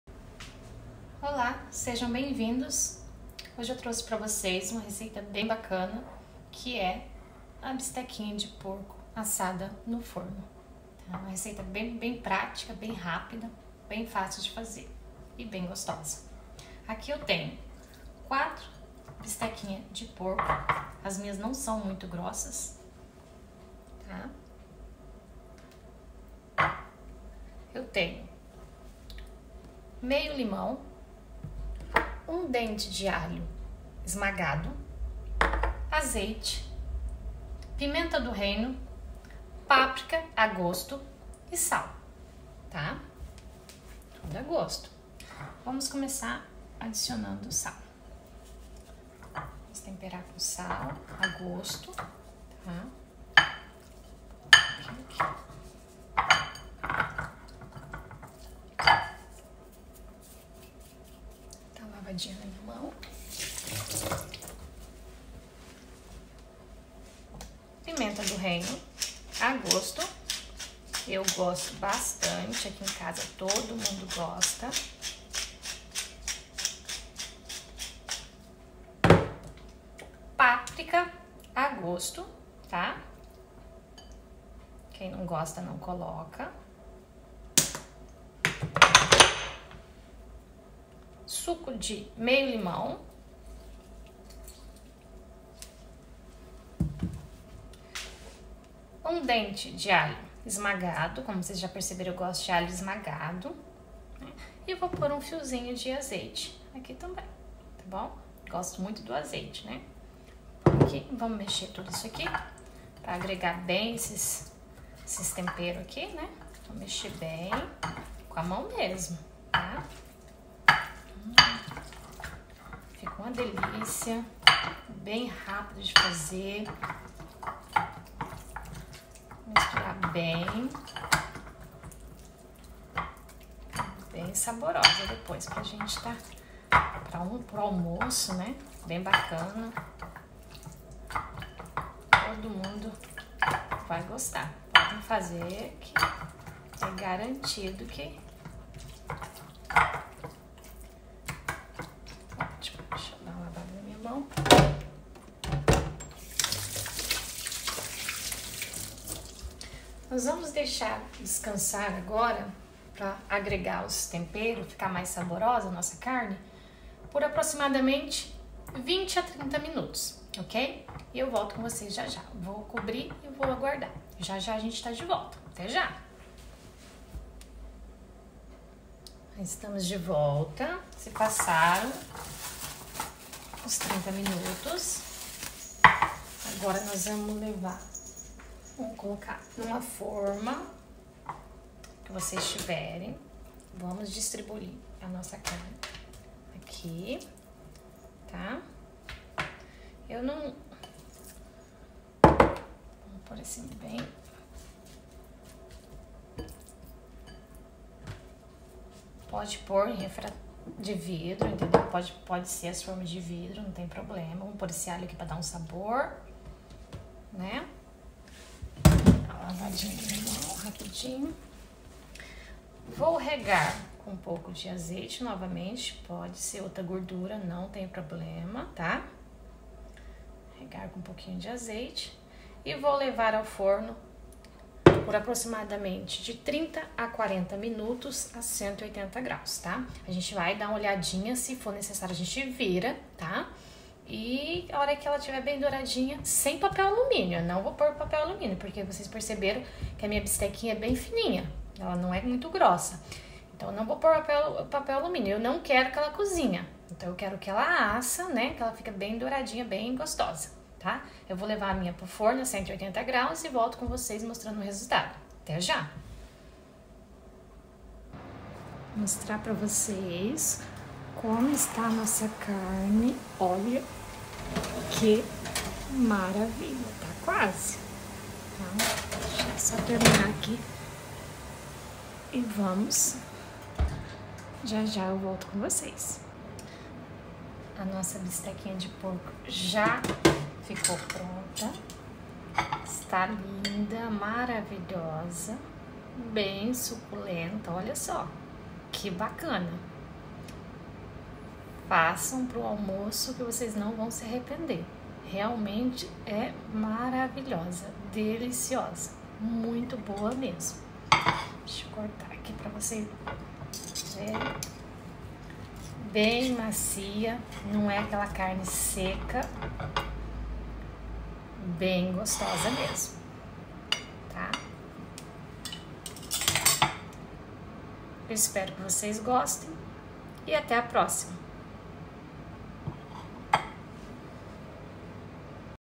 BISTECA DE PORCO NO FORNO ( super macia) AlesMenu
Bisteca de porco assada no forno fácil de fazer super macia e suculenta excelente receita muito saborosa uma delícia.